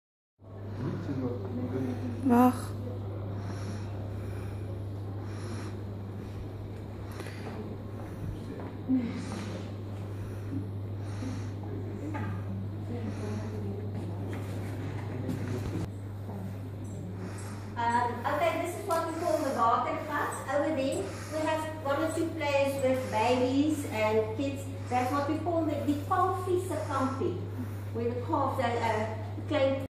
Vodacom Blue Bulls "Die Watergat"
Family area of Vodacom Bulls where players, wives and kids mingle before, during and after matches.